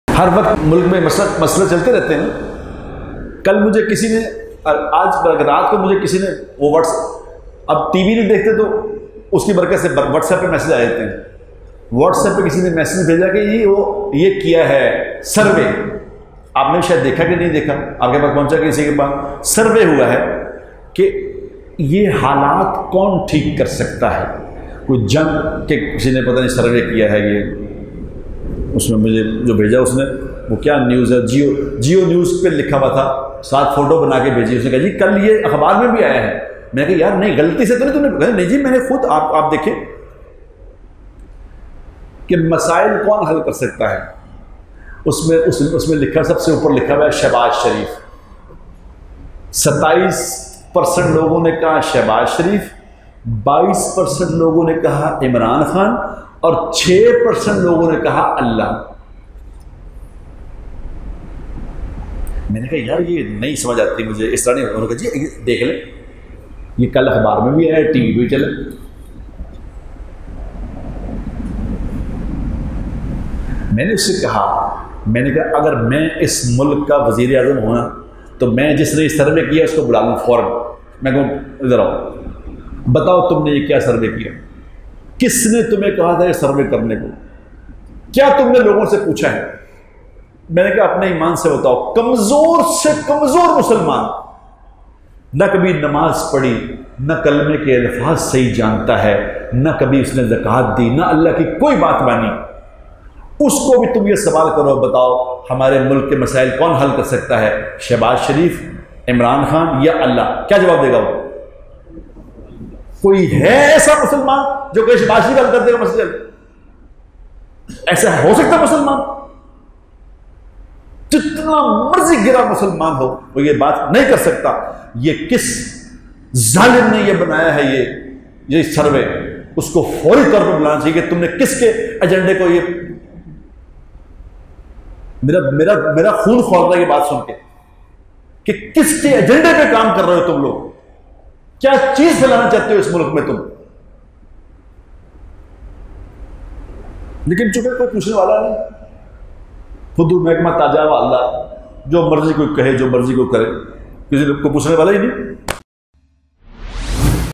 Shameful Survey by Geo News - Shahbaz Sharif , Imran khan or ALLAH !!! Naeem Butt Angry
جیو نیوز کا ایسا شرمناک سروے کہ آپ بھی نعیم بٹ صاحب کی طرح غصے میں آجائیں ۔
سروے میں لوگوں سے سوال پوچھا گیا کہ کون سی شخصیت پاکستان کو درپیش مسائل کو حل کرسکتی ہے ؟ اور جواب کے طور پر شہباز شریف ،عمران خان ، بلاول بھٹو ، مولانا فضل الرحمن ، سراج الحق  کے ناموں کے ساتھ اللہ کی ذات کا نام لیا گیا ! ۔